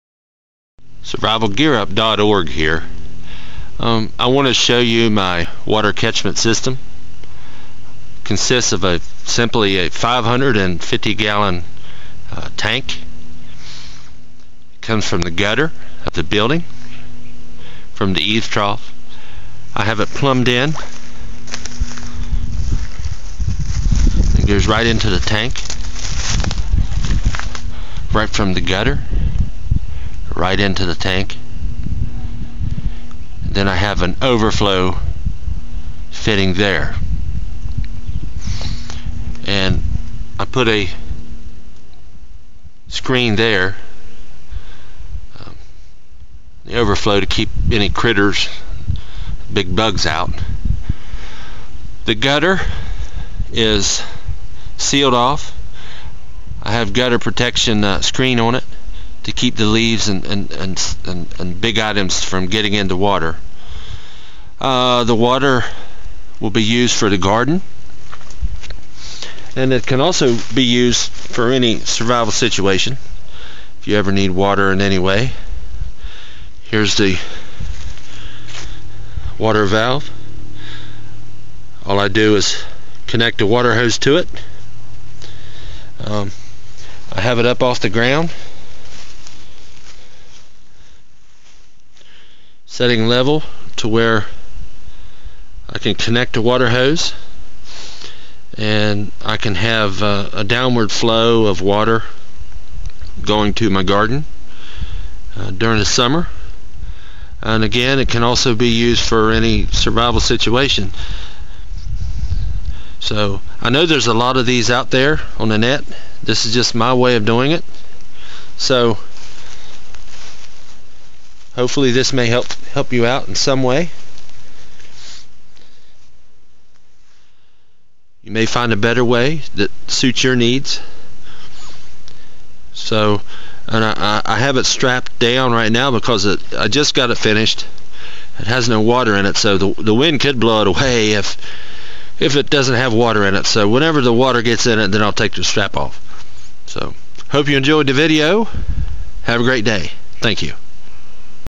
Water catchment system
This is my rain water catchment system for the garden.  Also very useful for any survival situation when in need of water.  It is a 550 gallon tank plumbed into the gutter system.  I have gutter guards on the gutter to keep out debris.  There is also an overflow pipe installed so water can escape when too full.  The overflow could also be attached to another tank if needed.